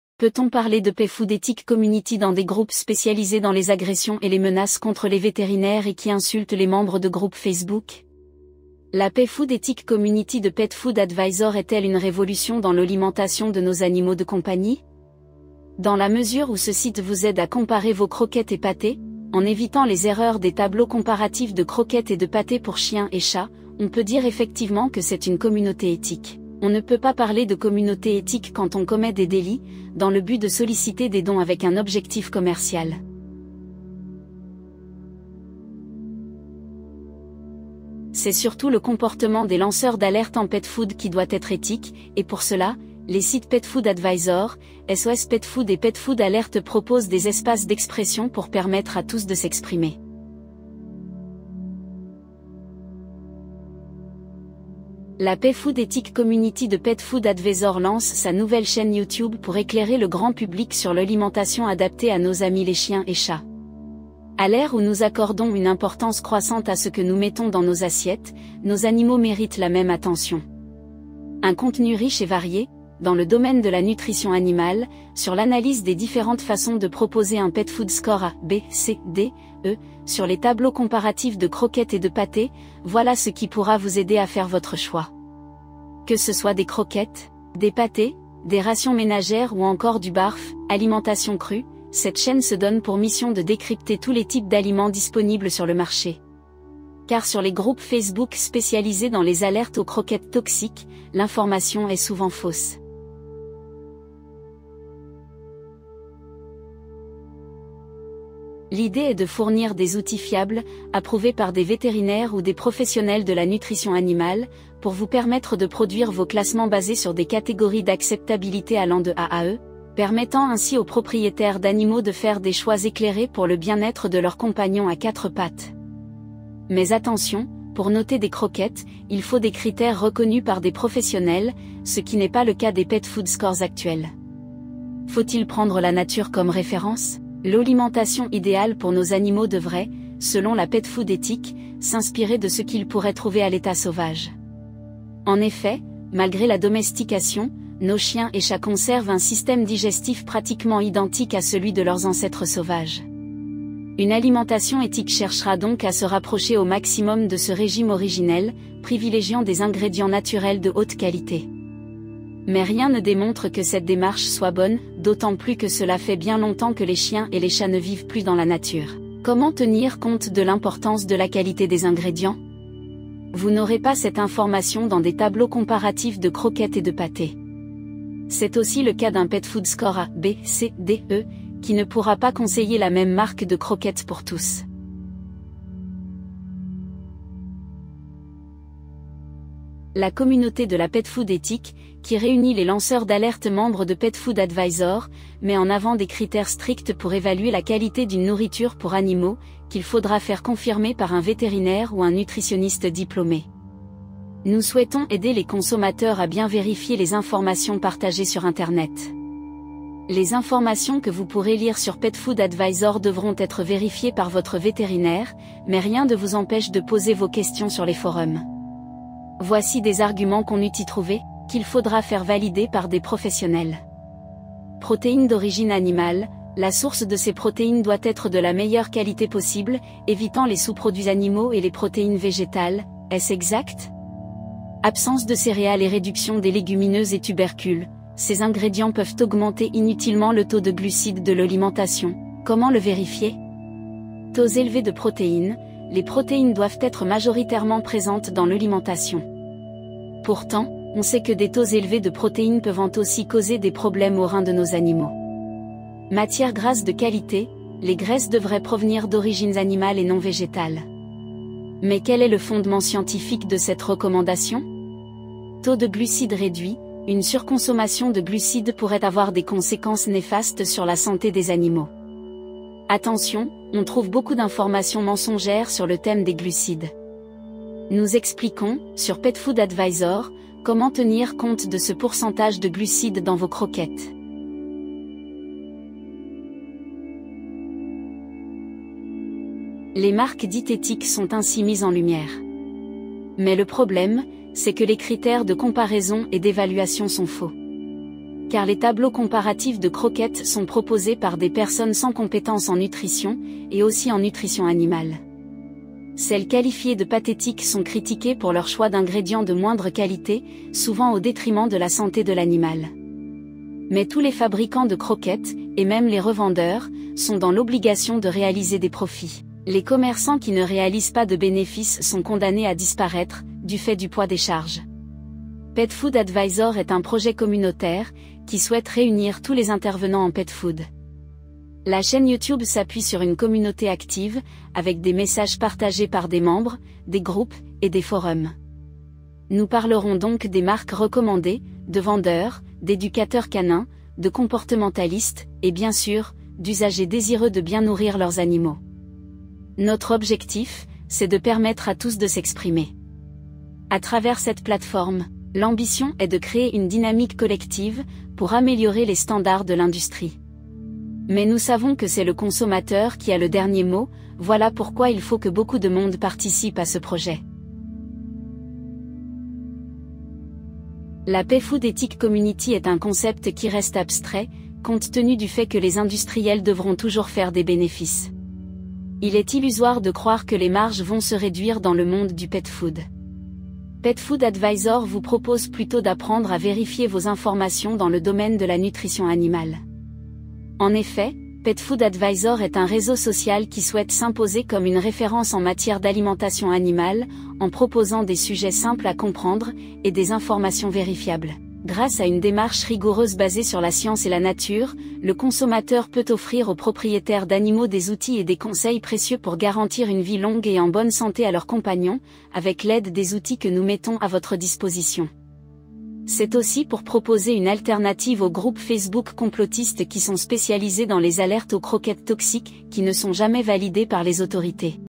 Qu'est-ce-que la Petfood Ethique Community Petfood Advisor ?
Dans les questions du forum, on nous a demandé si nous faisons partie de la Petfood Ethique Community. En fait, avant de parler d'éthique, il faut évoquer le comportement de ceux qui s'expriment sur le sujet du Petfood.

Mais dans la mesure où nous modérons les propos des membres, nous pouvons dire que vos contributions seront éthiques, car elles tiendront compte du bien-être de la communauté.

Les industriels auront toujours un objectif de rentabilité, il n'y a pas de Petfoodeur éthique. Prenez l'exemple du Nutriscore : des pommes de terre à frire sont classées A au Nutriscore ! C'est bien la preuve qu'un Petfood Score ne sert à rien, car les industriels vont contourner les systèmes d'évaluation pour proposer ce que demande le public.

Avec la mode du sans céréales, vous avez maintenant des croquettes qui ne sont pas forcément meilleures, mais qui s'adaptent à un mouvement.

Les vétérinaires "éthiques" qui écrivent sur les dangers des croquettes vendent des croquettes. Les vendeurs de croquettes sans céréales ont un objectif de rentabilité, ce qui est parfaitement normal, mais ils ne sont pas désintéressés. Voilà pourquoi il est difficile de parler d'éthique en Petfood.

Posez vos questions en commentaire, sur les groupes et les forums Petfood Advisor, sur cette définition de la Petfood Ethique.